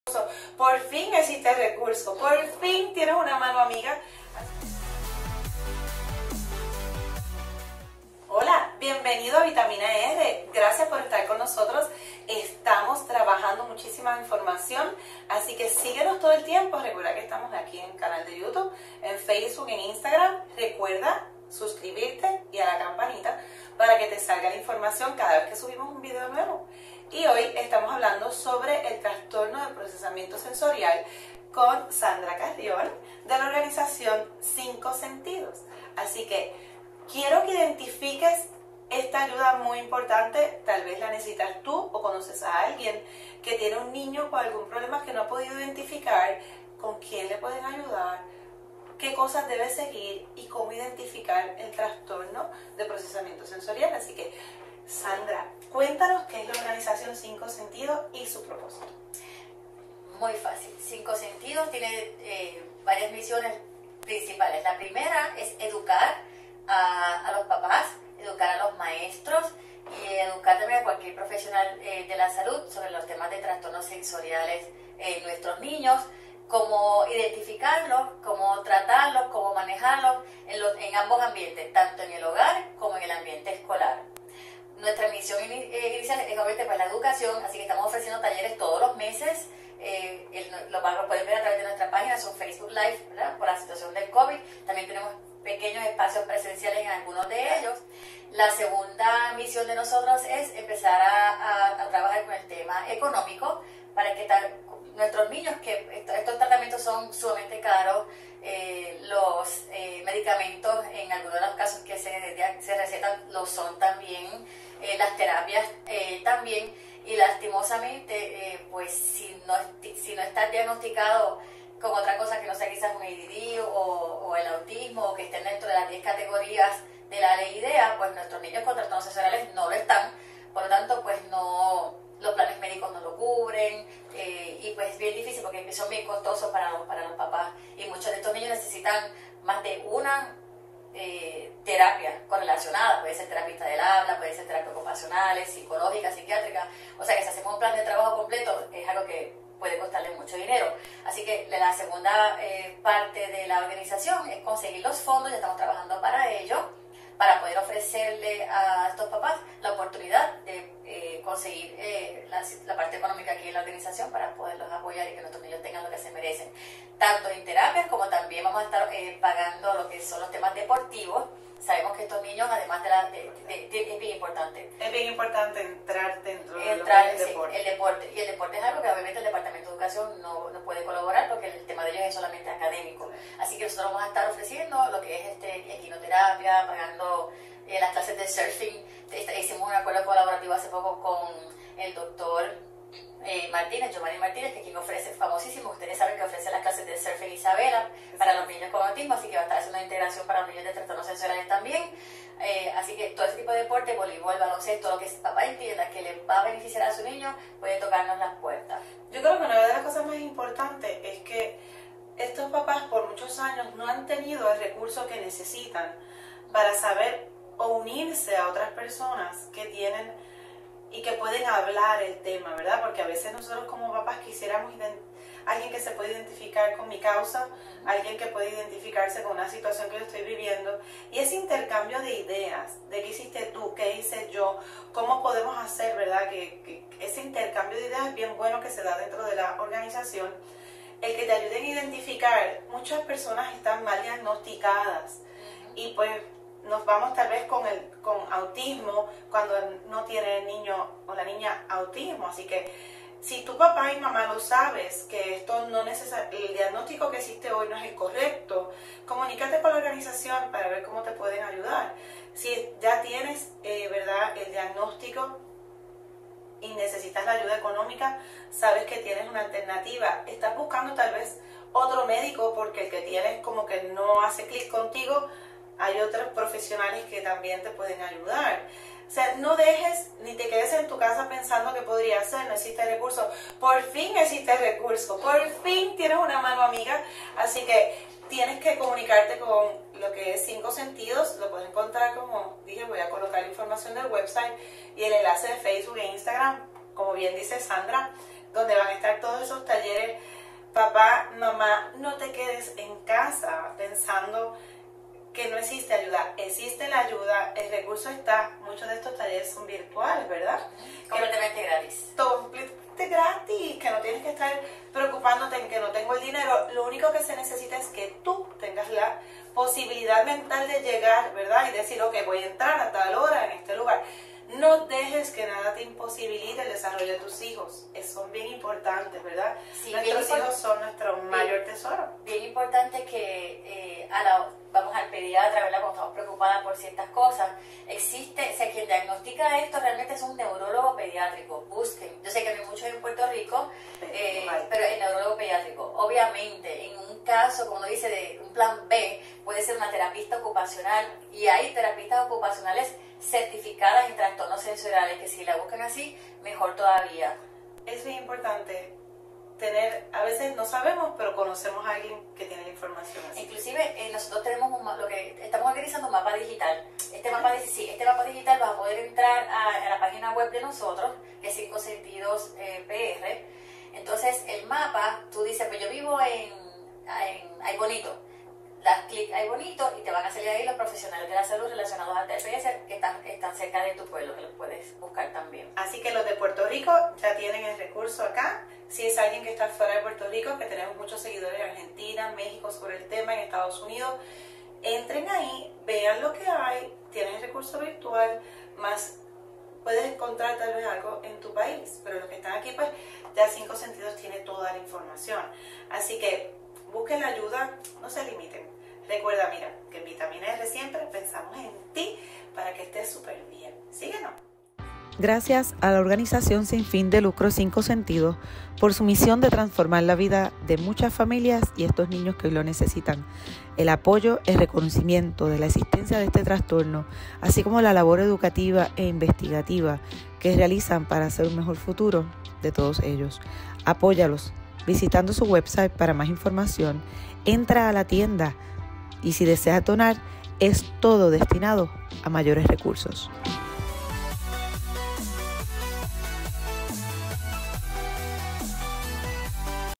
Tratamientos y recursos para integrar a niños con trastornos sensoriales (TPS)
Como padres y familiares preocupados siempre buscamos lo mejor para nuestros niños y si tienen trastornos sensoriales más. Sandra Carrión lo vive con sus gemelas y ha podido lograr tener a sus niñas en el nivel educativo y progresivo que desea. Su legado y ayuda a estos niños se hace pasión al realizar la organización 5 Sentidos para ti.

Si ya sabes el diagnóstico o necesitas más información y ayuda para conseguir:
- profesionales (médicos, terapistas, tratamientos)
- actividades
- integrarte a un grupo dedicado
- colaborar

* Organización 5 Sentidos
Organización sin fin de lucro dedicada a educar, investigar, apoyar y proveer los recursos que requieren los niños y jóvenes con TPS y sus familias.
Teléfono en Puerto Rico: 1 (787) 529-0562

* VitaminaER
Soy Edda Rocca y me dedico a encontrar Soluciones a Problemas Comunes de Salud.